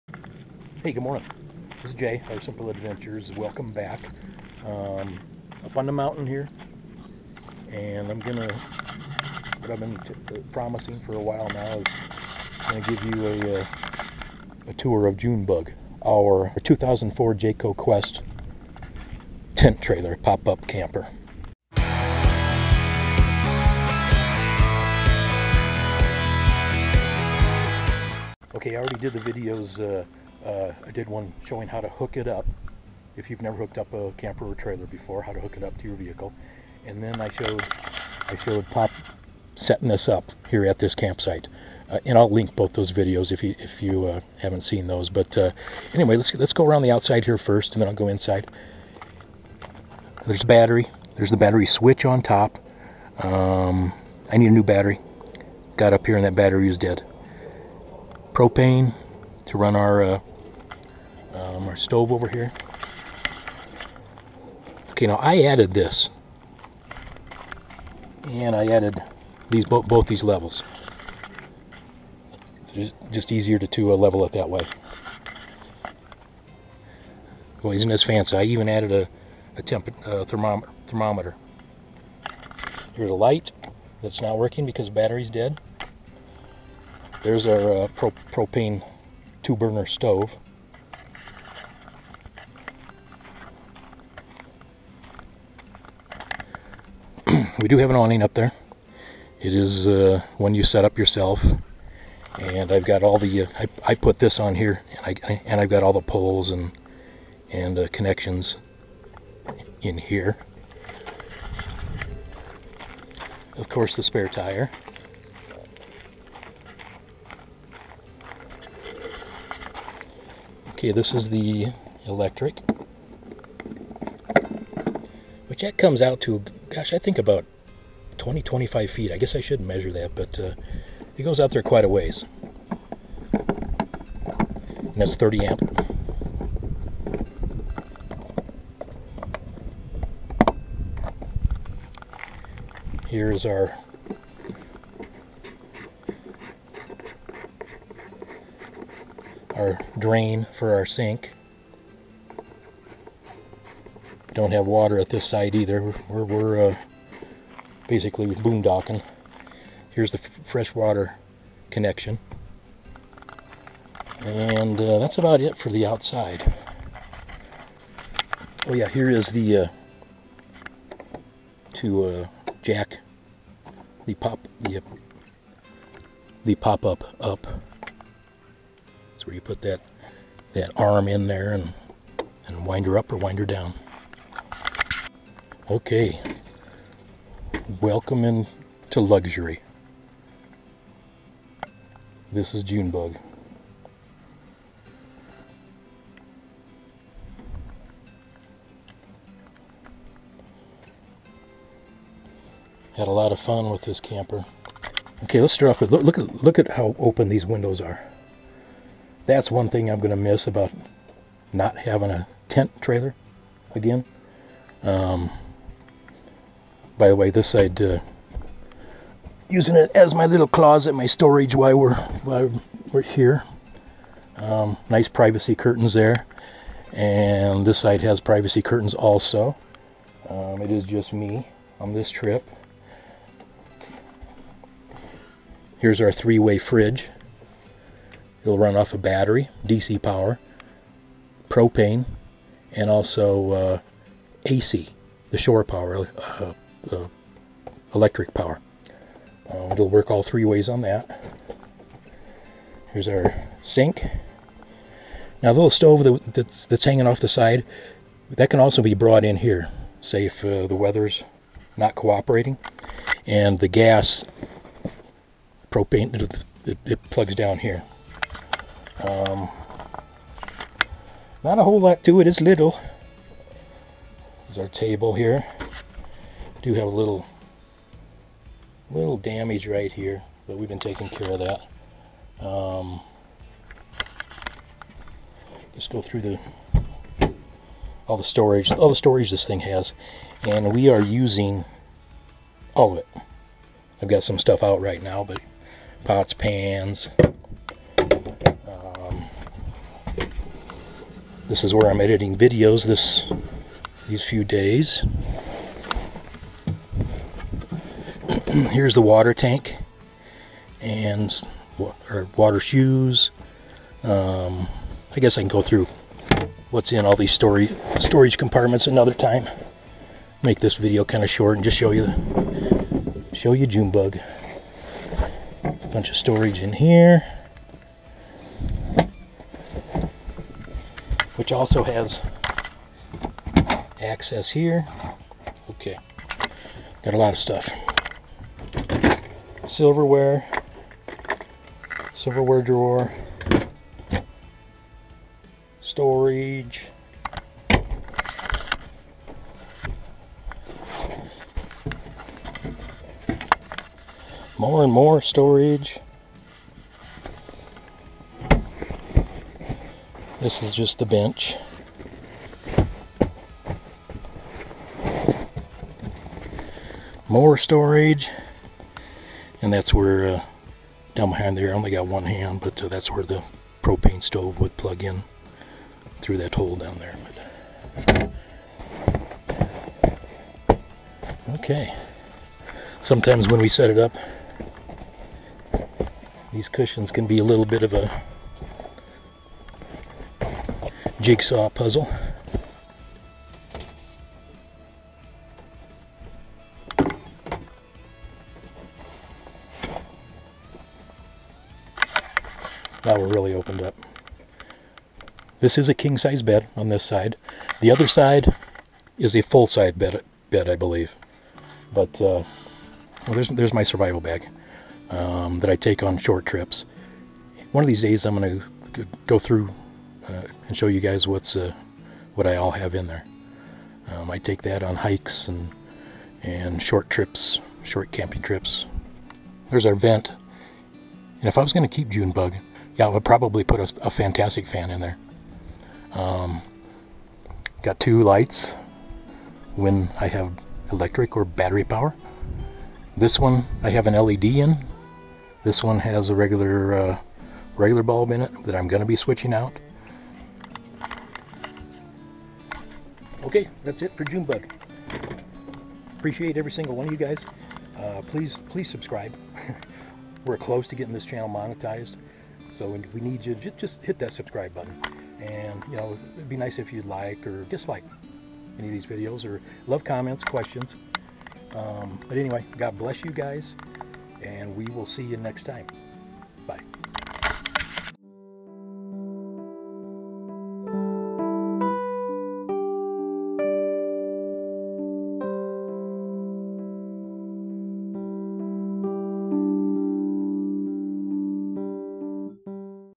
RV Tour of June Bug our Pop Up Tent Trailer. Camper For Sale?
The long awaited RV tour of our pop up tent trailer June Bug. Is our camper For Sale?